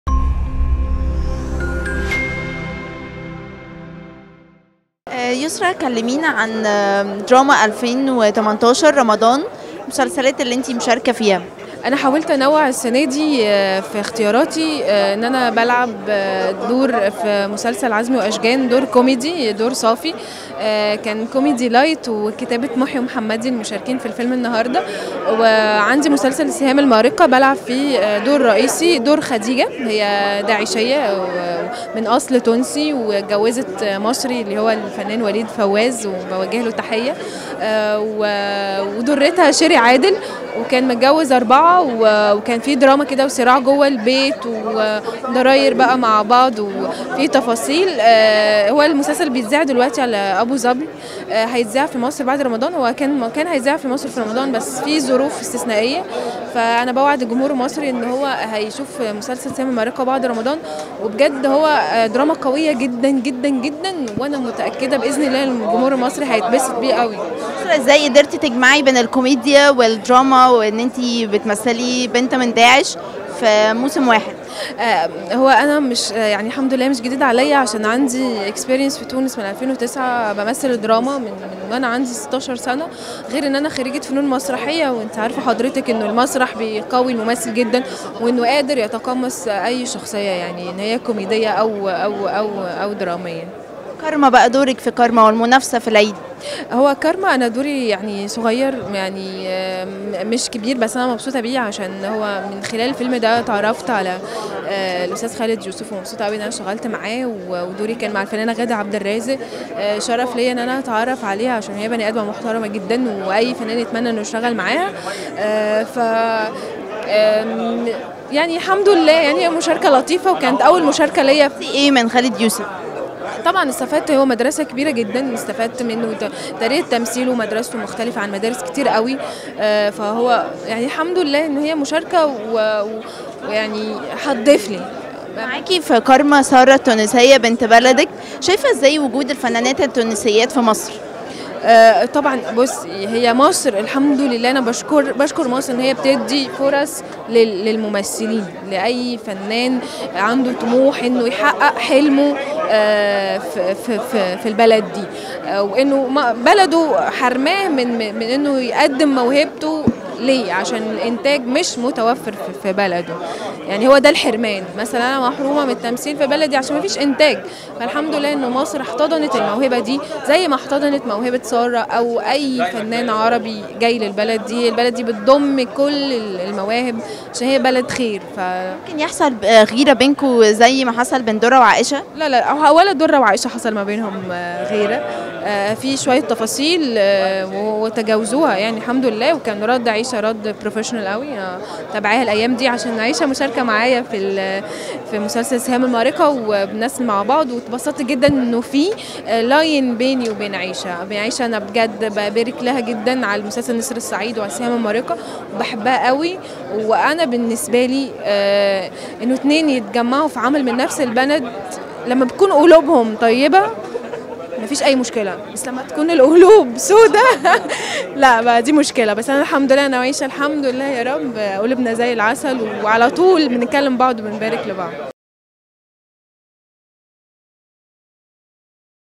يسرا المسعودي:رد عائشة بن أحمد على أزمتها مع درة كان احترافيا!
أطلت الفنانة التونسية يسرا المسعودي على جمهورها مؤخرًا في أكثر من دور مختلف، فظهرت بدور كوميدي في مسلسل "عزمي وأشجان" مع حسن الرداد وإيمي سمير غانم، وفي دور تراجيدي درامي إنساني بمسلسل "السهام المارقة" مع نخبة من كبار النجوم، كما أطلت بدور مختلف في فيلم "كارما" للمخرج خالد يوسف.

وأعربت يسرا في لقاء خاص مع "فوشيا" عن سعادتها بالعمل في مصر مع باقي زميلاتها الفنانات التونسيات مثل درة وسارة، وعائشة بن أحمد، اللاتي يشاركن في أكثر من عمل، وكشفت عن طريقة تعاملها معهن حتى لا تحدث أي أزمة كما حدث بين درة وعائشة في مسلسل "نسر الصعيد".

وأشارت الفنانة الشابة إلى قدرتها على التحول بين الأدوار الكوميدية والتراجيدية التي قدمتها خلال شهر رمضان هذا العام، وكيف حاولت أن تتنوع في أعمالها حتى تظهر موهبتها للجمهور العربي والمصري.

وتحدثت يسرا عن دورها في فيلم "كارما" للمخرج خالد يوسف، وبطولة الفنان عمرو سعد، وغادة عبدالرازق، وكيف استفادت من خبرتهم الكبيرة.